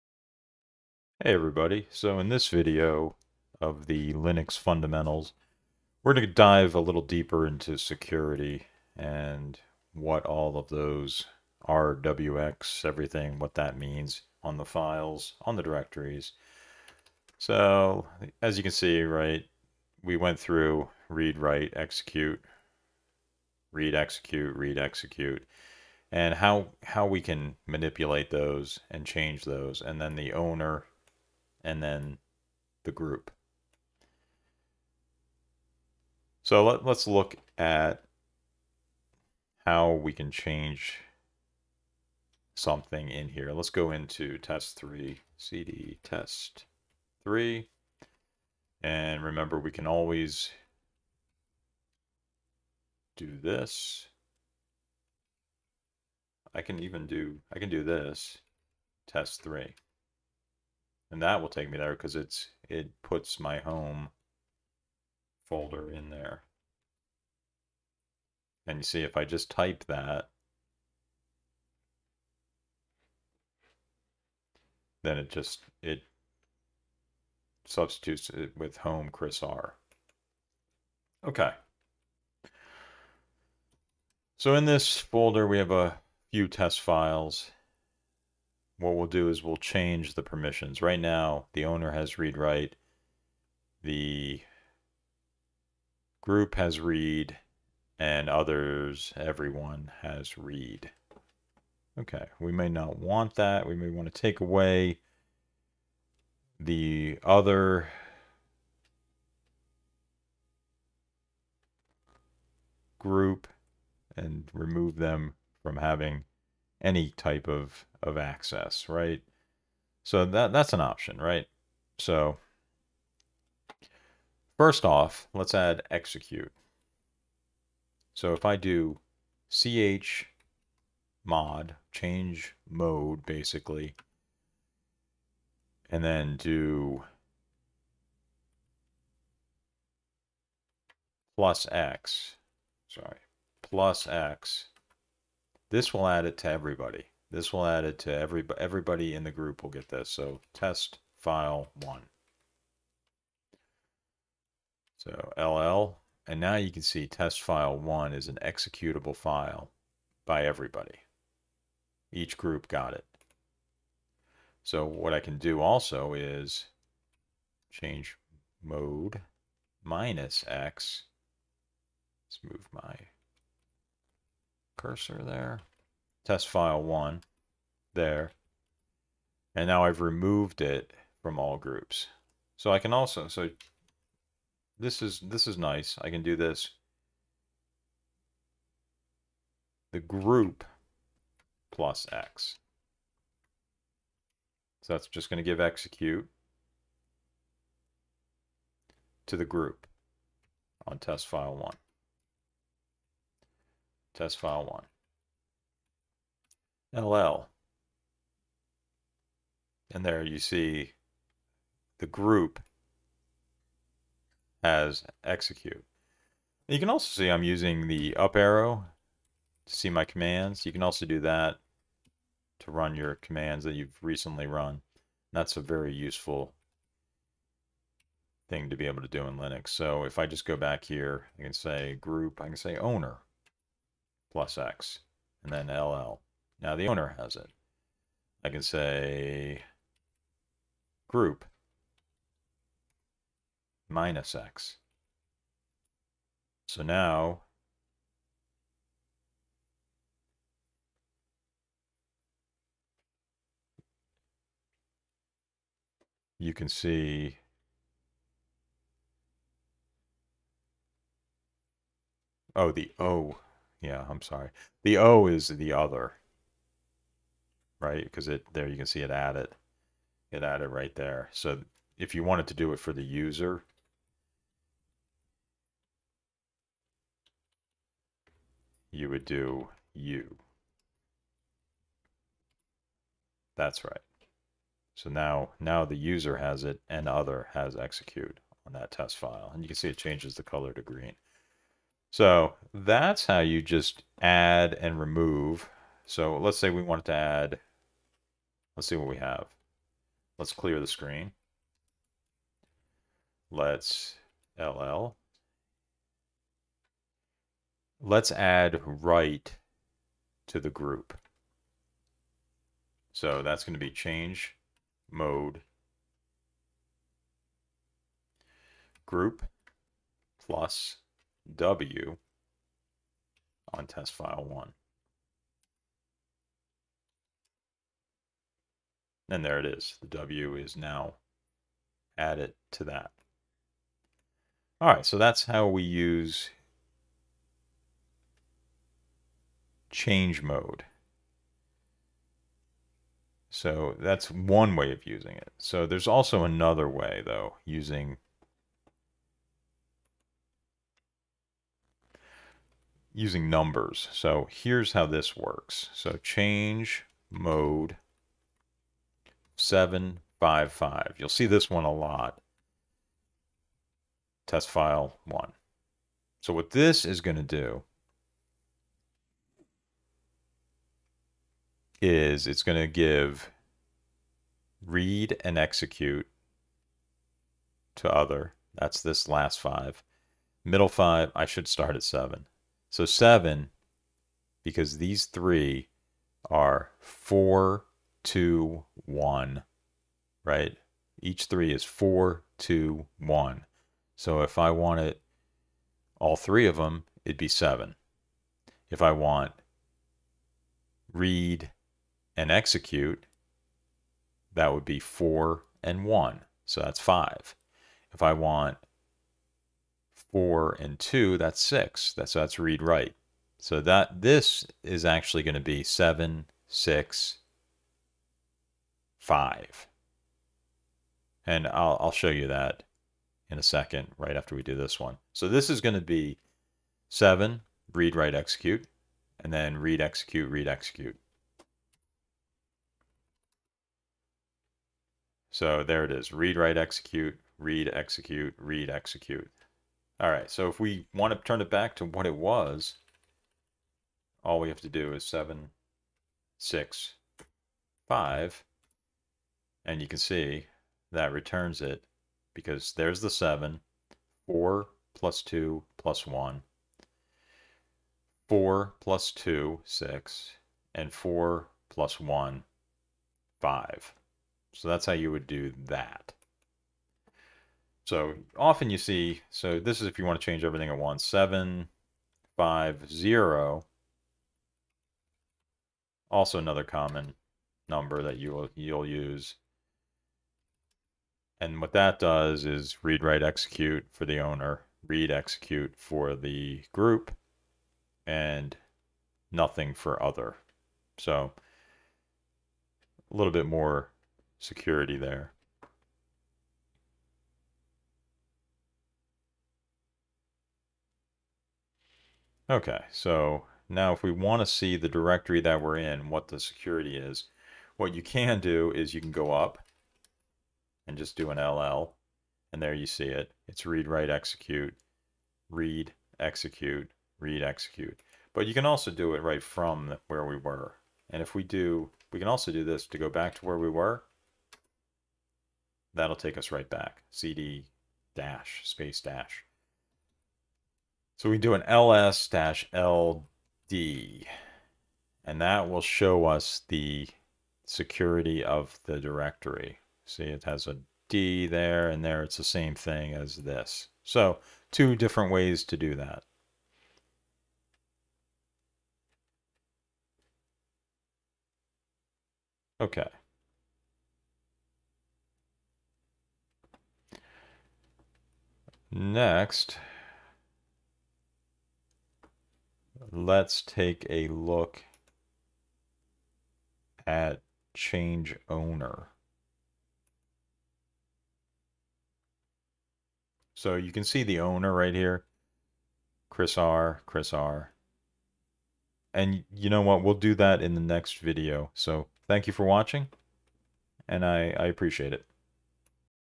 Intro to Linux - Rocky Linux 9 - Permissions management using chmod
Learn how to install and configure Rocky Linux in a VM.  Rocky Linux is built from RHEL source. This set of videos is perfect for someone new to Linux or looking to learn more about the latest versions.